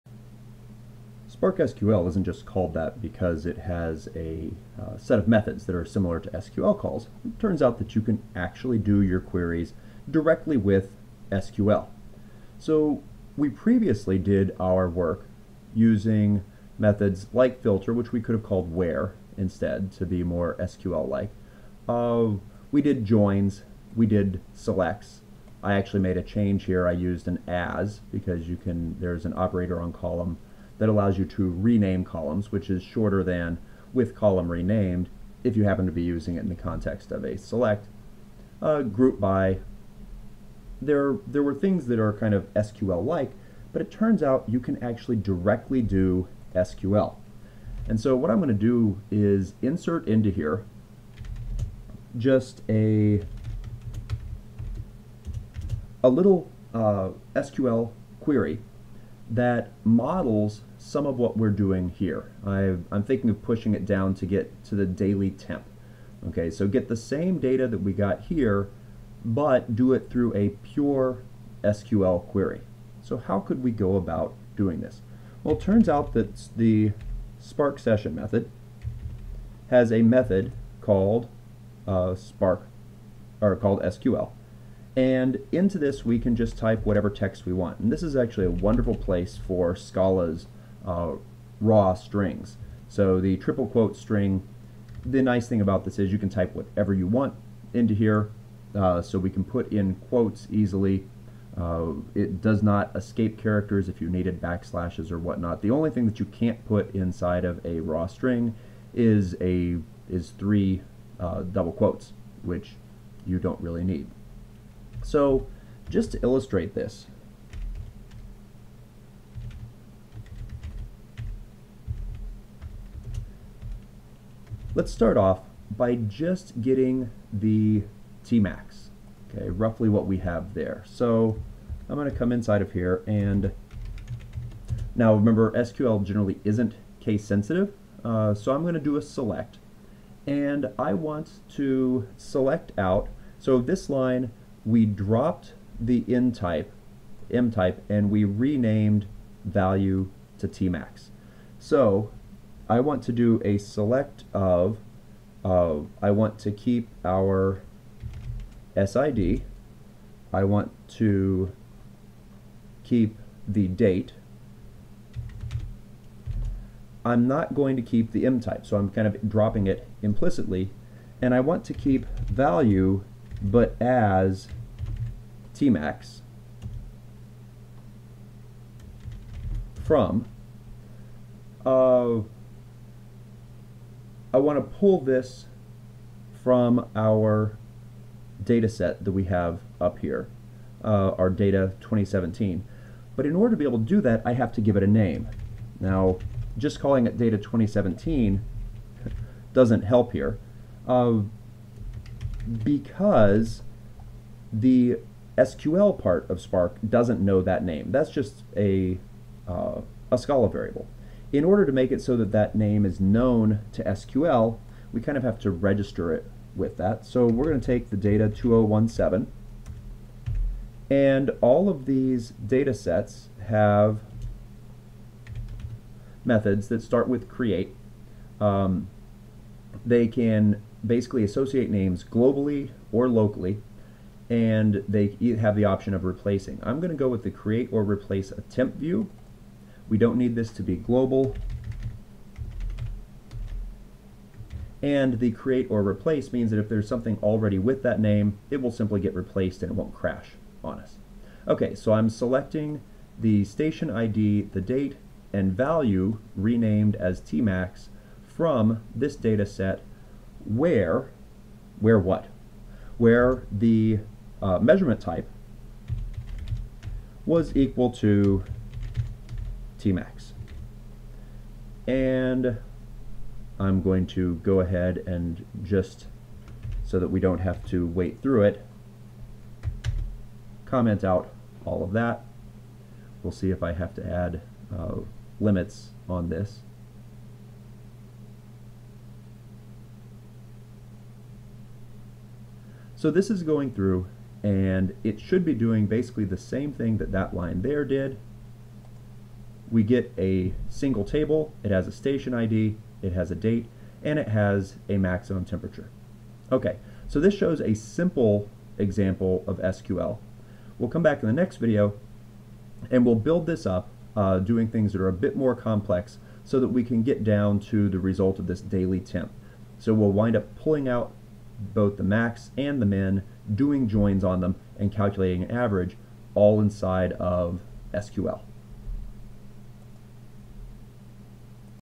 Spark SQL with SQL Part 1 (using Scala)
This video introduces how you can do Spark queries using text SQL.